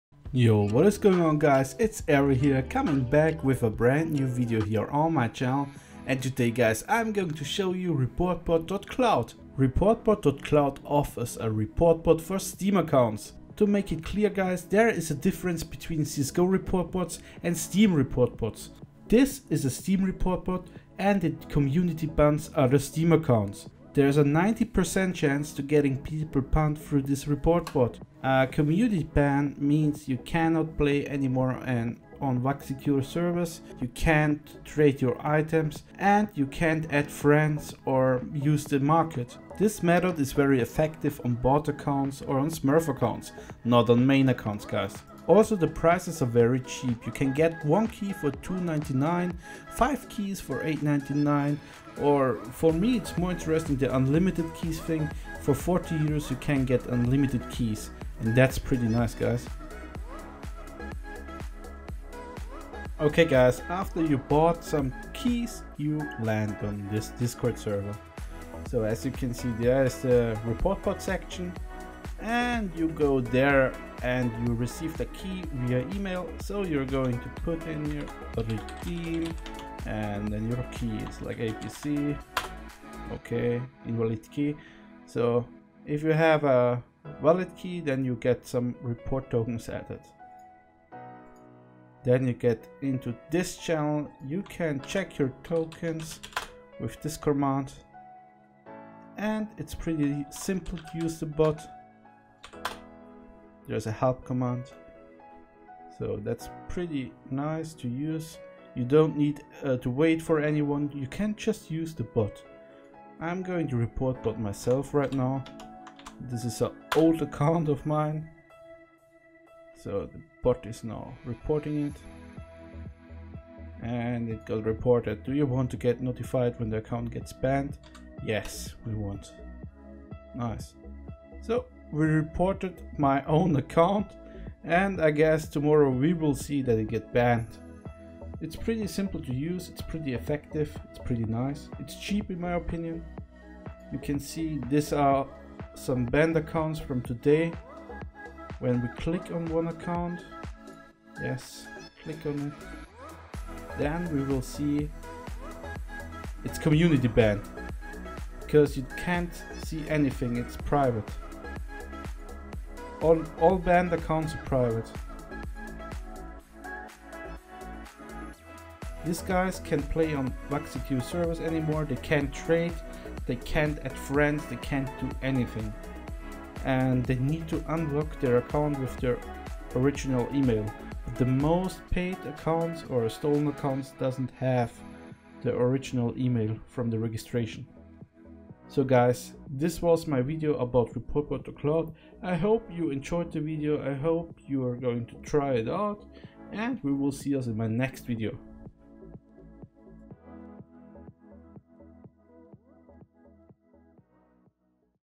STEAM REPORTBOT | COMMUNITY BAN OTHER STEAM ACCOUNTS | REPORTBOT.CLOUD | HIGH BANRATE
⚡️ STEAM REPORTBOT | COMMUNITY BAN OTHER STEAM ACCOUNTS ⚡️

In this video I will show you the best Steam Reportbot. With this Reportbot you can community ban other Steam accounts. Its pretty easy to use and it works pretty good. With this Steam Reportbot you can easy ban other players. It works perfect for smurf or bought accounts.